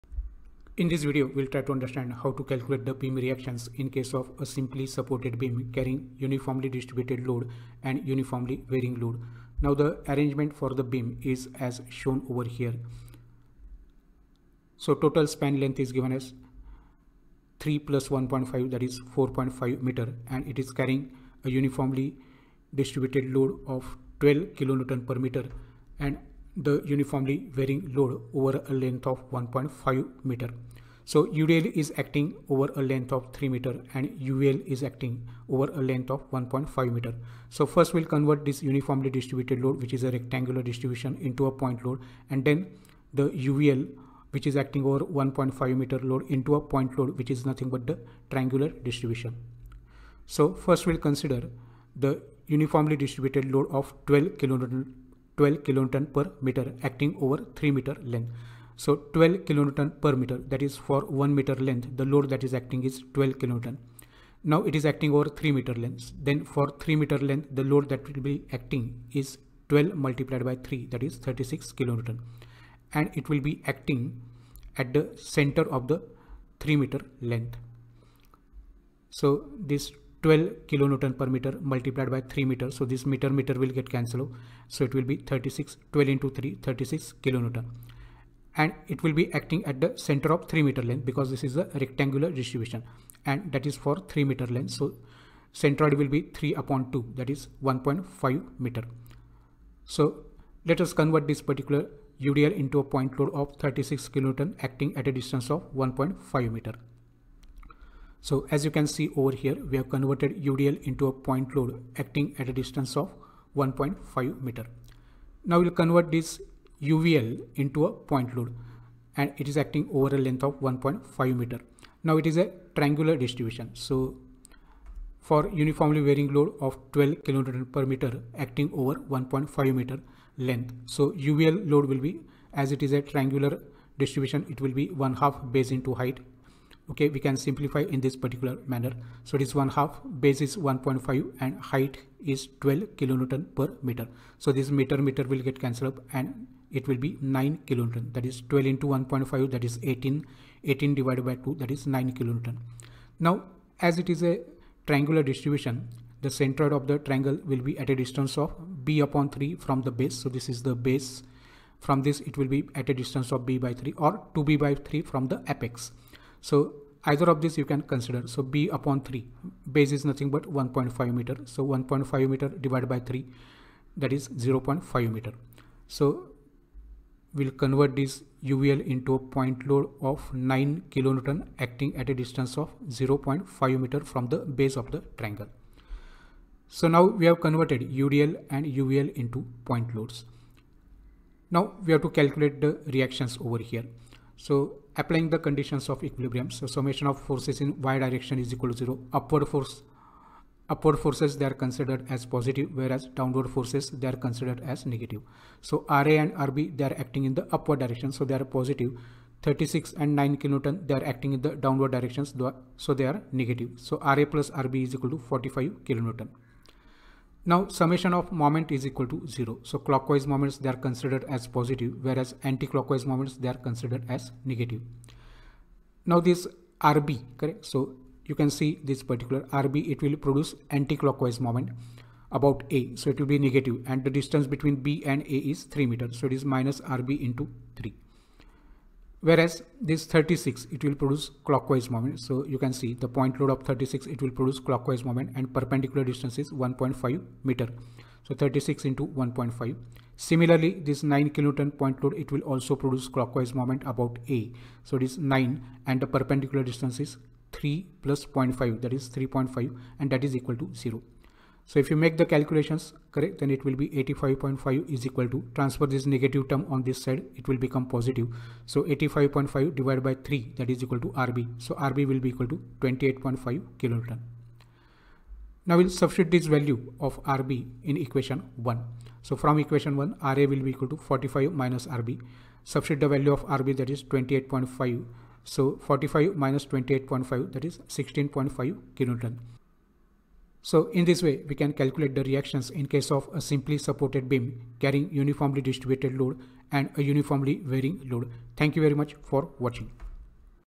Lecture 18 | How to calculate the beam reaction | Simply supported beam carrying u.d.l. & u.v.l.
This video explains how to calculate the beam reactions in case of a simply supported beam carrying uniformly distributed load(u.d.l.) & uniformly varying load (u.v.l.).
Beam reaction problem in Engineering Mechanics has great significance.
The beam reaction problems can be solved by using both analytical method as well as
graphical method. The graphical method used to solve the examples in Engineering
mechanics is also known as Graphic Statics.